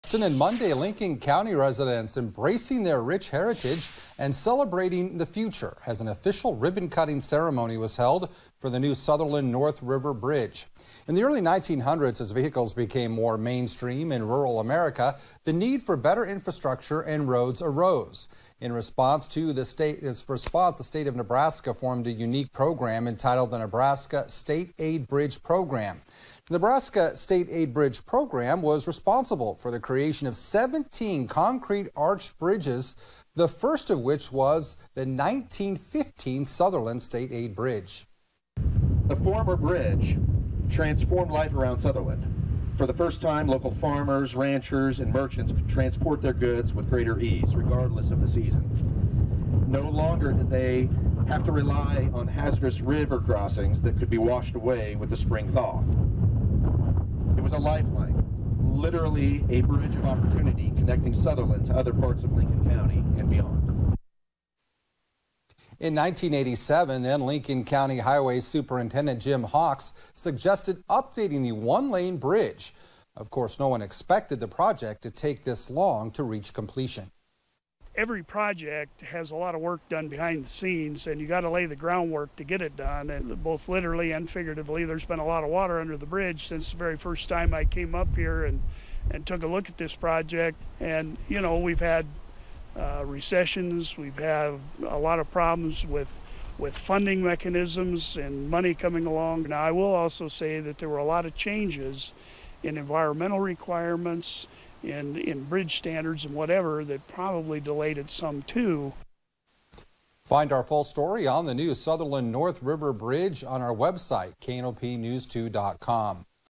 Lincoln County celebrates completion of Sutherland North River Bridge
The new bridge officially opened on August 30, 2024. One month later, current Lincoln County Commissioners held a ribbon-cutting ceremony